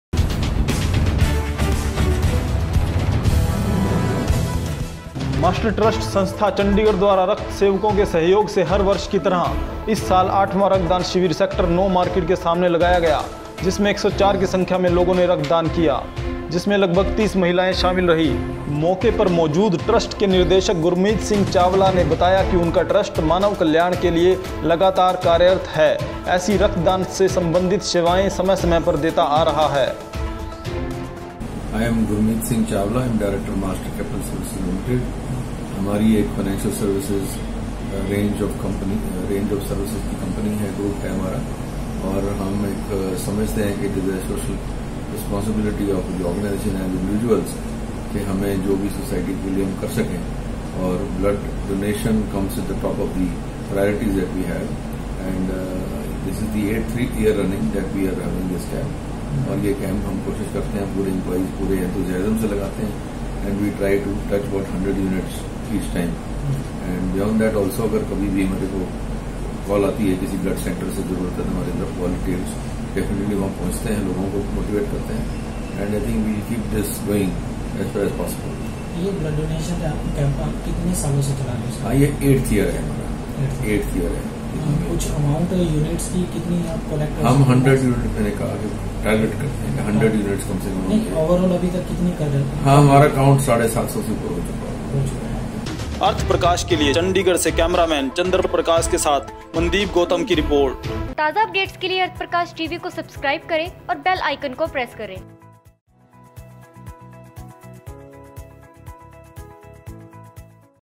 Chandigarh सेक्टर 9 मार्किट : 104 की संख्या में लोगों ने किया रक्तदान
मास्टर ट्रस्ट संस्था चंडीगढ़ द्वारा रक्त सेवकों  के सहयोग से हर वर्ष की तरह  इस साल आठवां रक्तदान शिविर सेक्टर 9 मार्किट के सामने लगाया गया जिसमें 104 की संख्या में लोगों ने रक्तदान किया जिसमें लगभग ३० महिलायें शामिल रही मौके पर मौजूद ट्रस्ट के निर्देशक गुरमीत सिंह चावला ने बताया कि उनका ट्रस्ट मानव कल्याण के लिए लगातार कार्यरत है ऐसी रक्तदान से संबंधित सेवायें समय समय पर देता आ रहा है